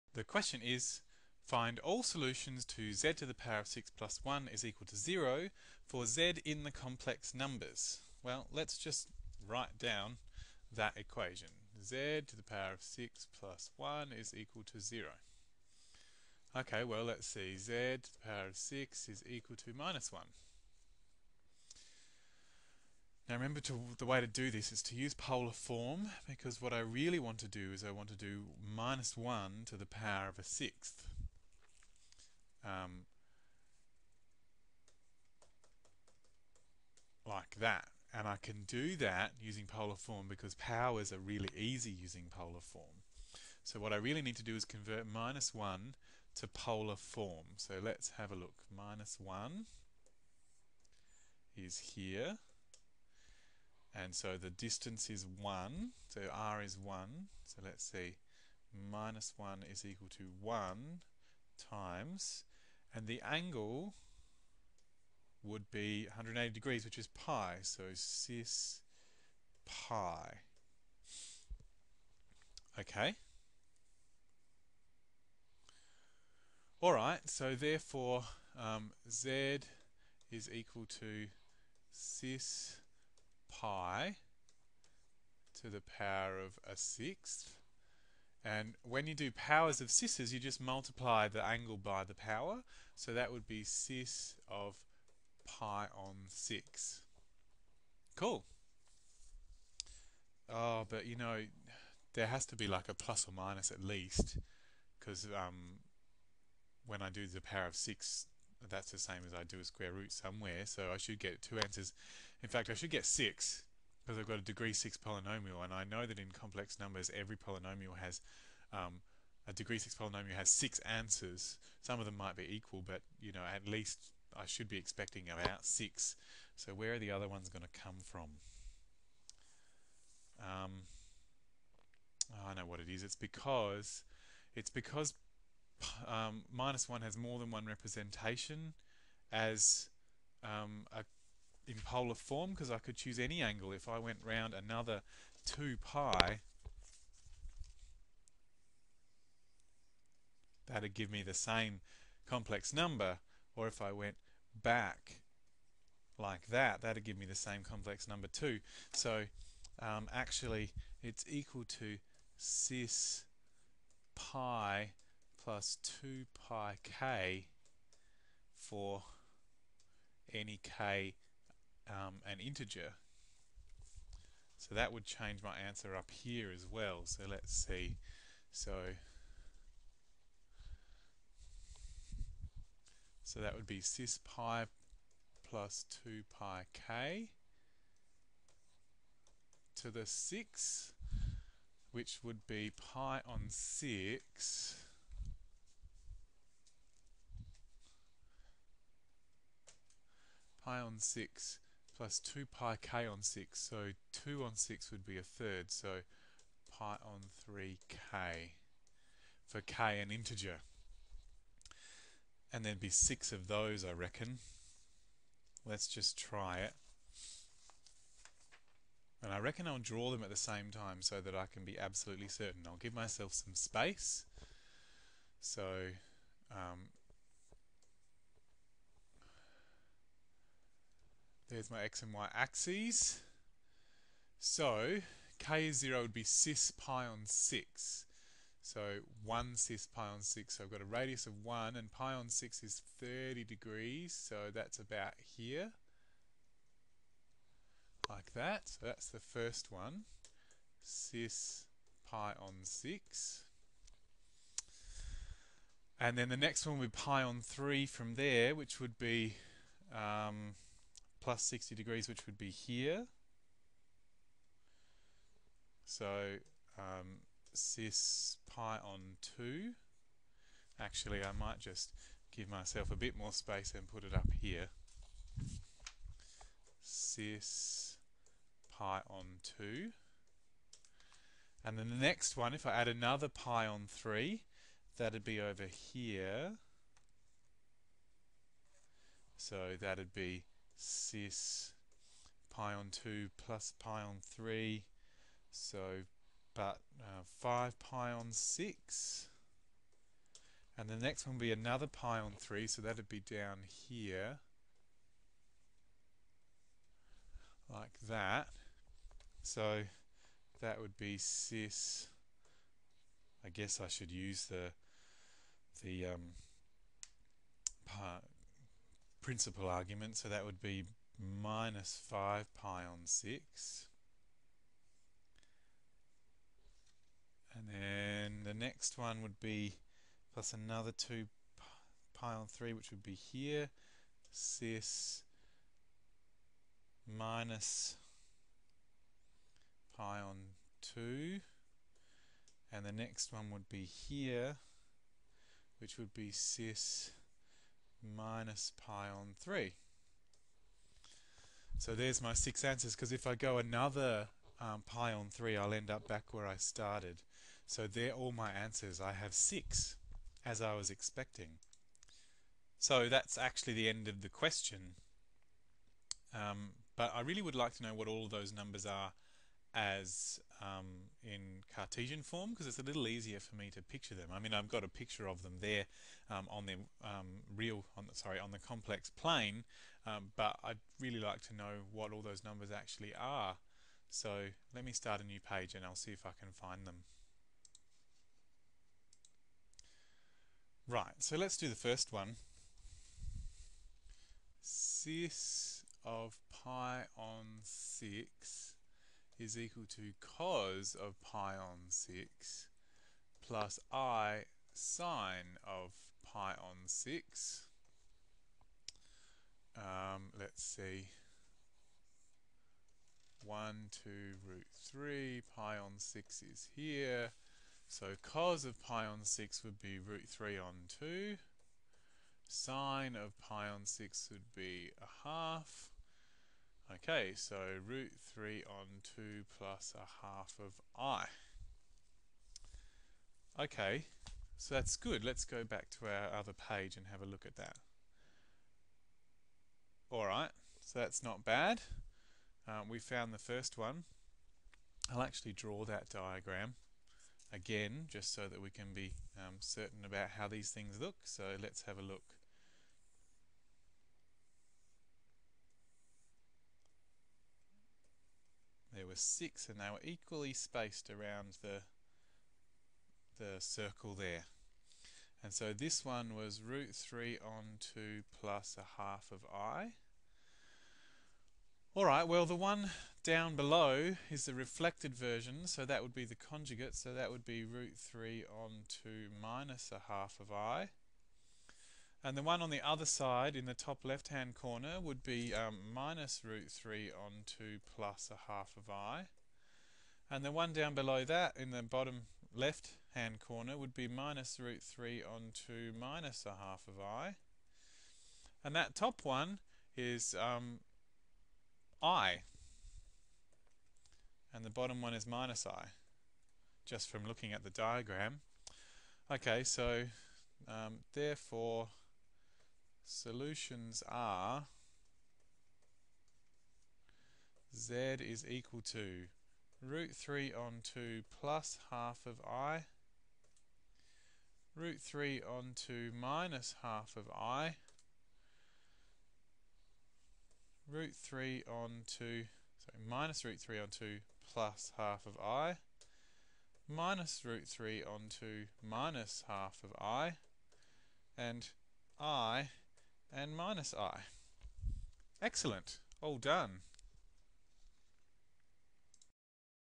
EXAMPLE: Finding all the sixth roots of a complex number
Worked example by David Butler. Features finding all the solutions to an equation, involving finding the sixth roots of -1.